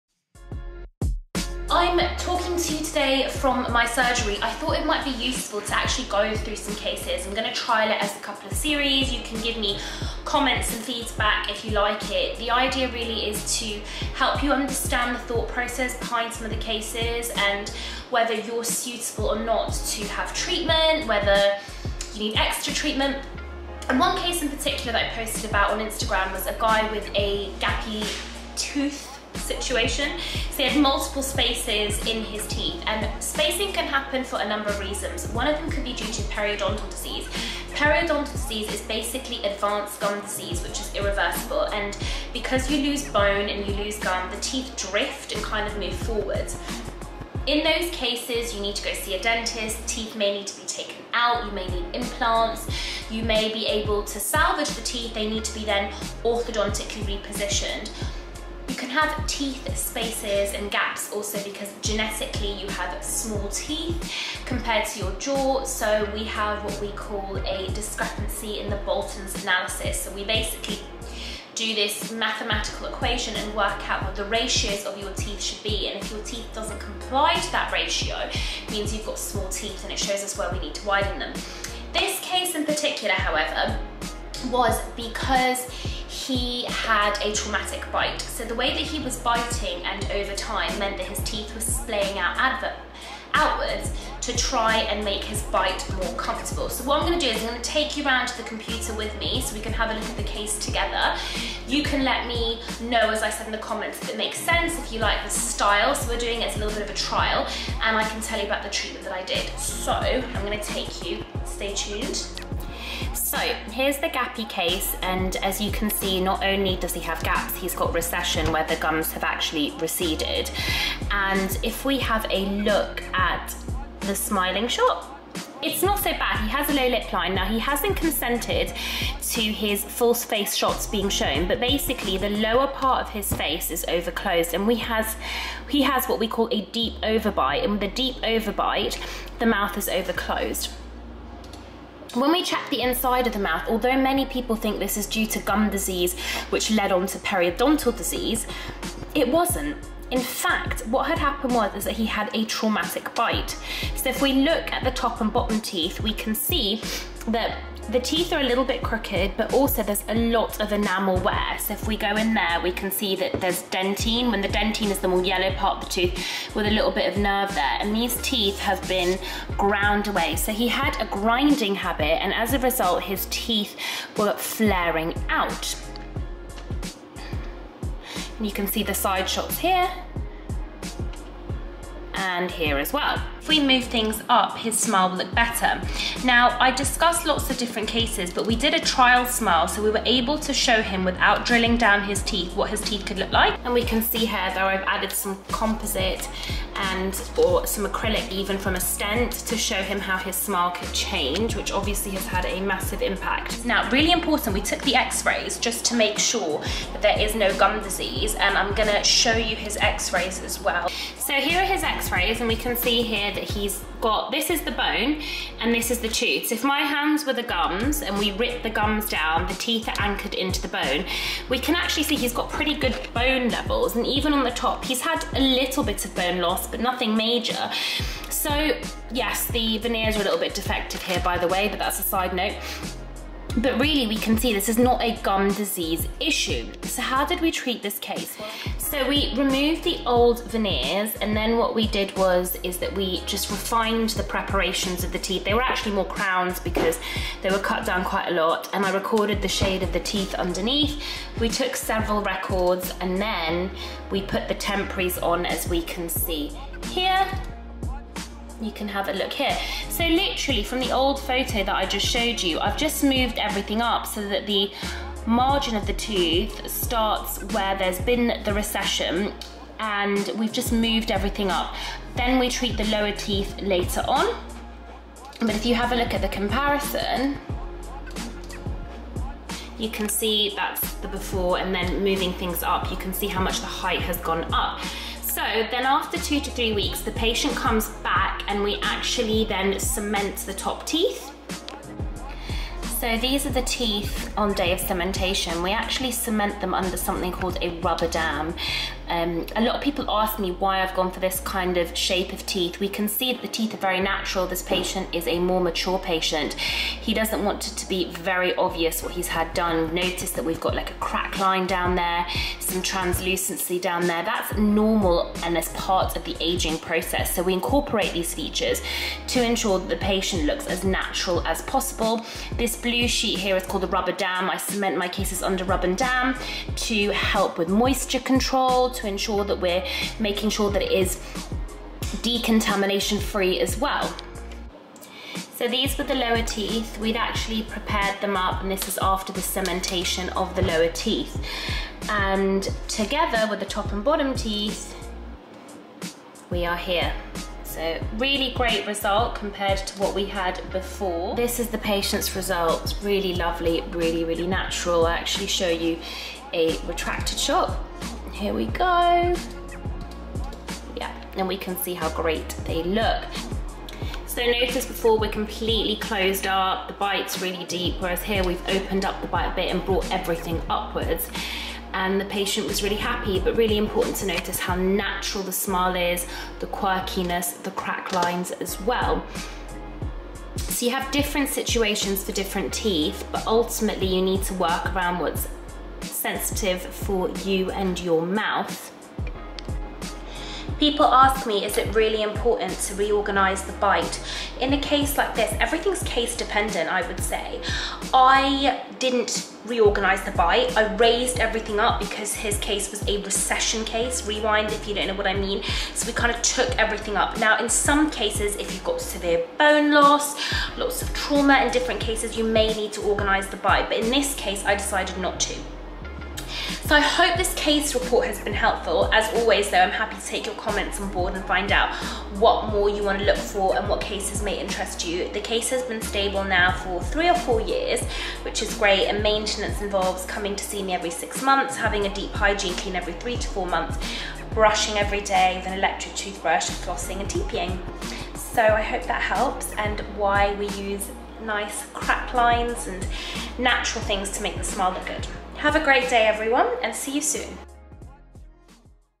Case Discussion with Dr Rhona - Gappy Teeth
In todays video, I talk you through a recent patient case of mine who had a Gappy Smile, that they wanted to change and how I was able to achieve their dream smile.

I will be doing a series of these discussions to help you understand the thought process behind some of the cases that I do and for you to know whether you are suitable or not to have treatment, or if you need further treatment before hand.

Spaces or gaps in teeth can happen for a few reasons:
- Periodontal disease (advanced gum disease), where teeth drift and move forward
- Genetically

Let me know if you enjoy this style of video and case walk through, as well as any feedback you might have.